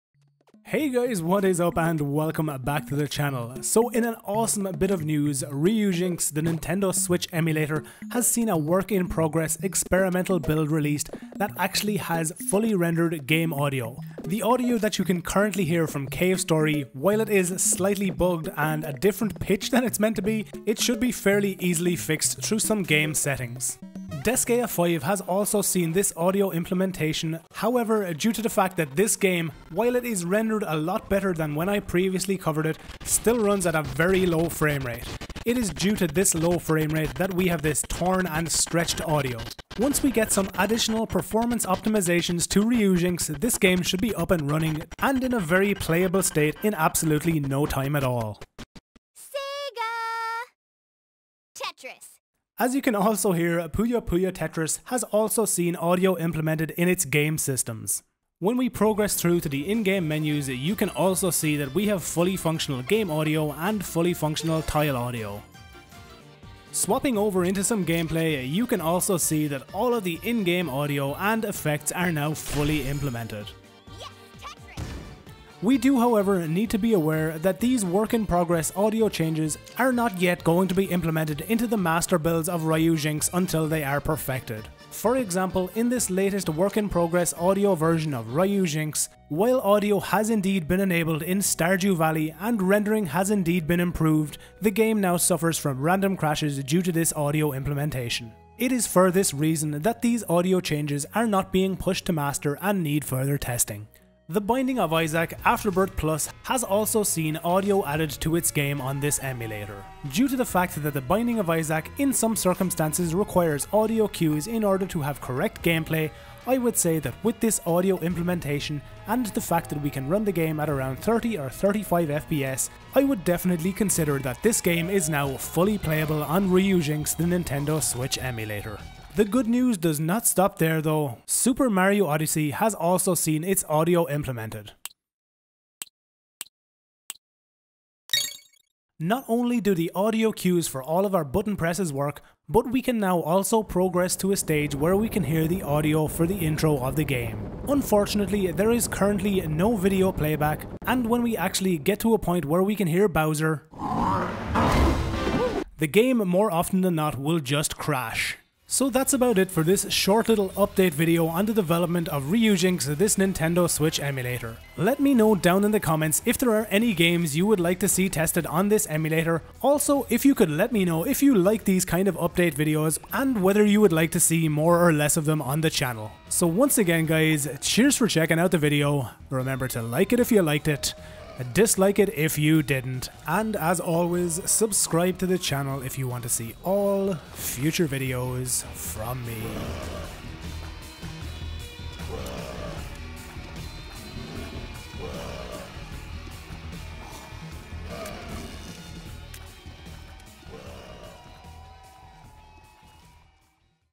Ryujinx | Switch Emulator gets a HUGE Upgrade | New Audio Renderer
Ryujinx has seen an Experimental Audio Renderer added, which gives us audio support in lots of our games.

► My PC Specs:
16gb DDR4 3200Mhz RAM
GTX980ti 6gb
256gb NVME M.2 SSD